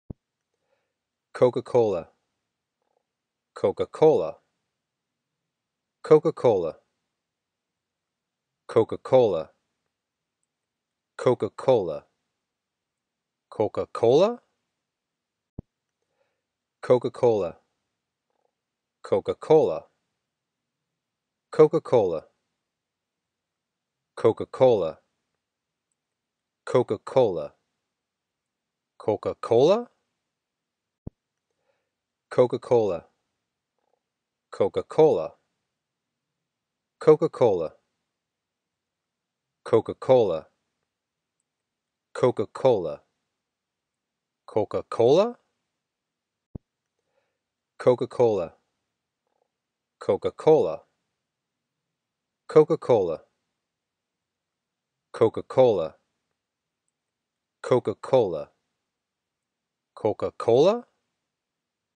How to Pronounce Coca Cola In english
This video was made by a native english speaker to help you pronounce the word, Coca Cola. Please by sure to look at my other pronunciation videos for more help. Thank you.